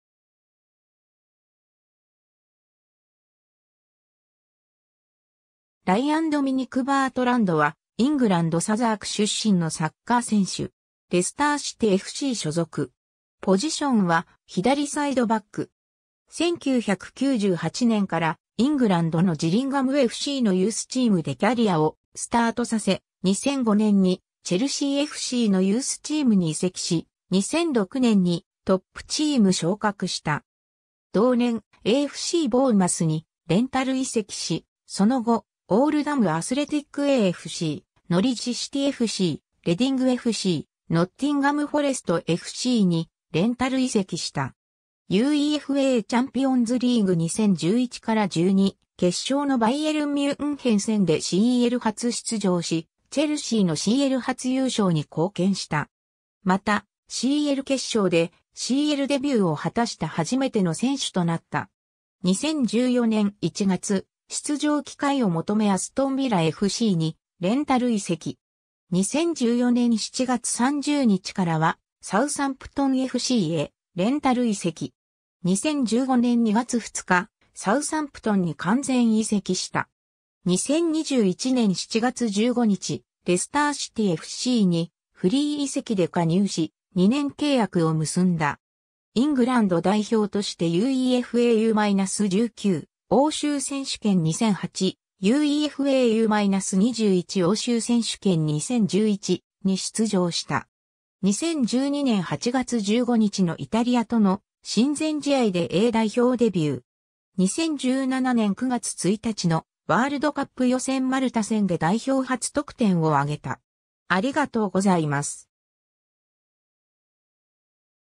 ライアン・バートランド
オールダム・アスレティックAFCの選手
ライアン・ドミニク・バートランド（Ryan Dominic Bertrand, 1989年8月5日 - ）は、イングランド・サザーク出身のサッカー選手。
レスター・シティFC所属。
ポジションは左サイドバック。
 1998年からイングランドのジリンガムFCのユースチームでキャリアをスタートさせ、2005年にチェルシーFCのユースチームに移籍し、2006年にトップチーム昇格した。
同年、AFCボーンマスにレンタル移籍し、その後、オールダム・アスレティックAFC、ノリッジ・シティFC、レディングFC、ノッティンガム・フォレストFCにレンタル移籍した。
 UEFAチャンピオンズリーグ 2011-12 決勝のバイエルン・ミュンヘン戦でCL初出場しチェルシーのCL初優勝に貢献した。
またCL決勝でCLデビューを果たした初めての選手となった。
 2014年1月、出場機会を求めアストン・ヴィラFCにレンタル移籍。
2014年7月30日からはサウサンプトンFCへレンタル移籍。
2015年2月2日、サウサンプトンに完全移籍した。
 2021年7月15日、レスター・シティFCにフリー移籍で加入し、2年契約を結んだ。
 イングランド代表としてUEFA U-19欧州選手権2008、UEFA U-21欧州選手権2011に出場した。
 2012年8月15日のイタリアとの親善試合でA代表デビュー。
2017年9月1日のワールドカップ予選マルタ戦で代表初得点を挙げた。